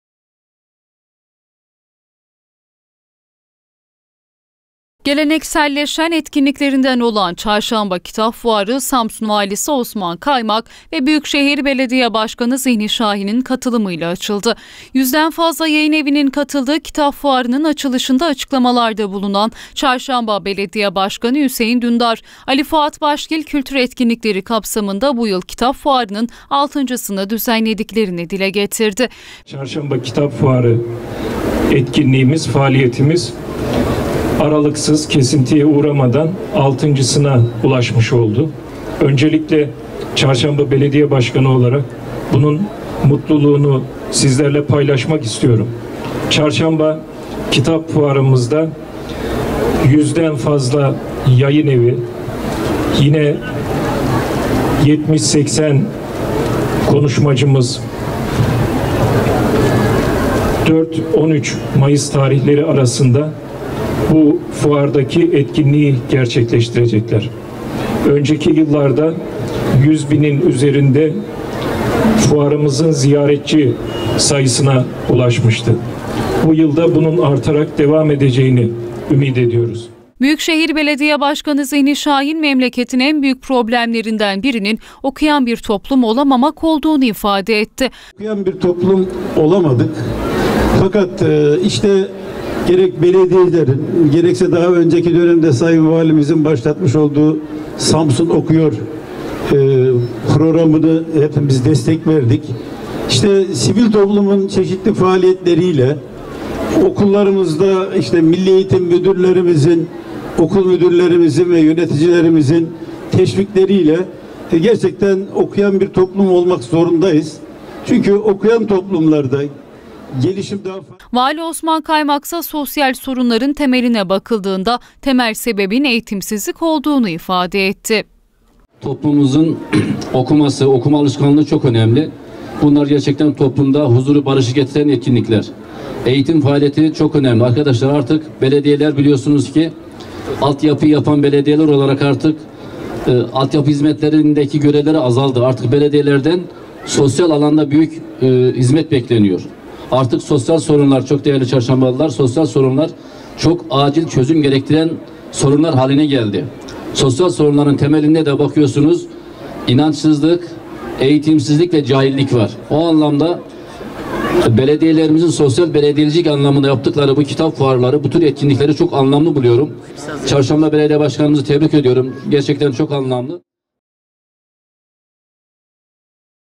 6. Çarşamba Kitap Fuarı açıldı
6. Çarşamba Kitap Fuarı'nın açılışında konuşan Belediye Başkanı Hüseyin Dündar, "Fuar sayesinde ilçemizdeki öğrencileri kitapla buluşturuyoruz" dedi